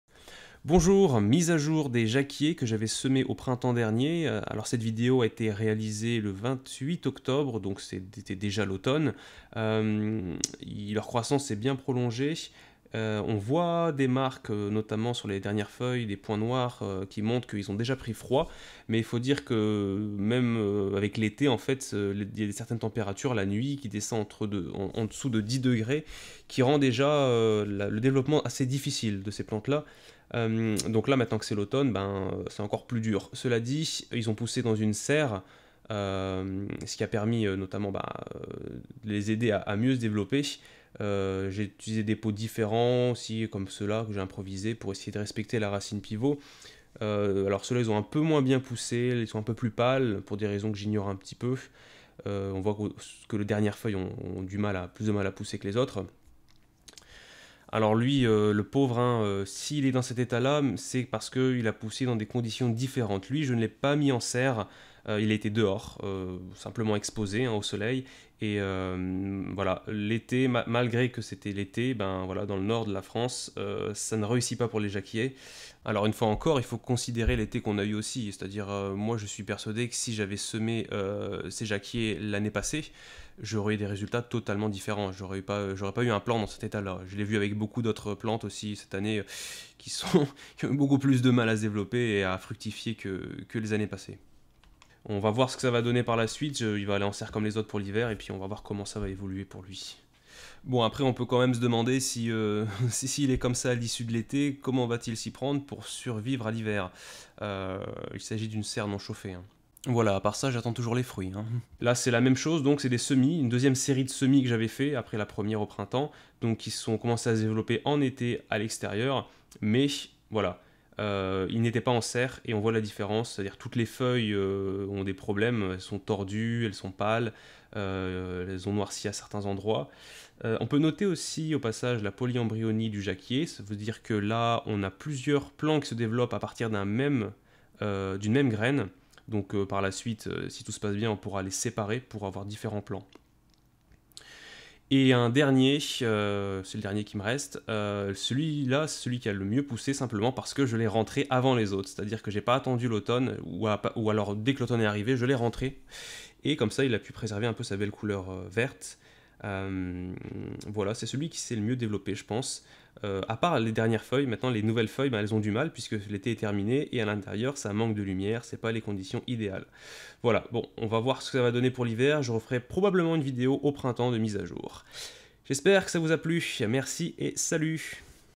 [MAJ] Les jacquiers à l'automne
Voici l'état d'avancement des plants de jacquier.
Vidéo réalisée le 28 octobre.